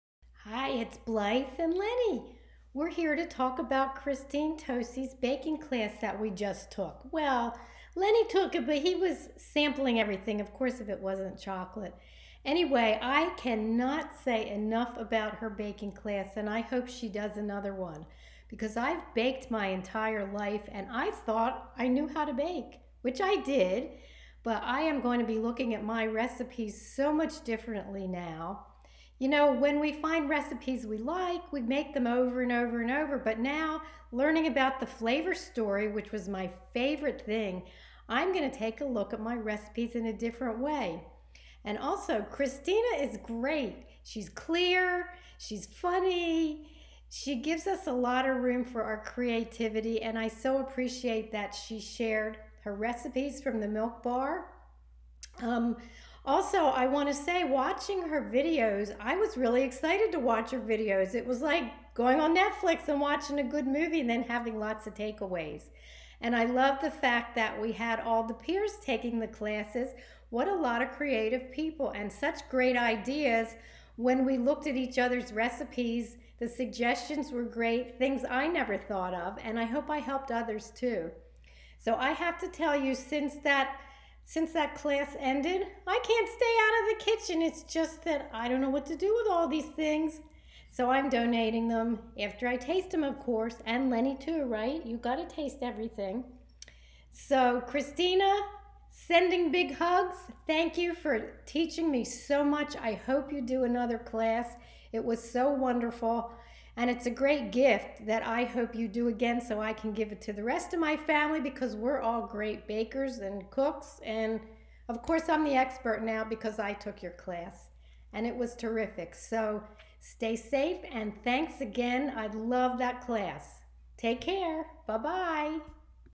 Christina Tosi from Milk Bar Class Review 2021 SD 480p
Christina Tosi from Milk Bar offered a wonderful baking class and I'm here to tell you about it! I can almost smell the cookies baking!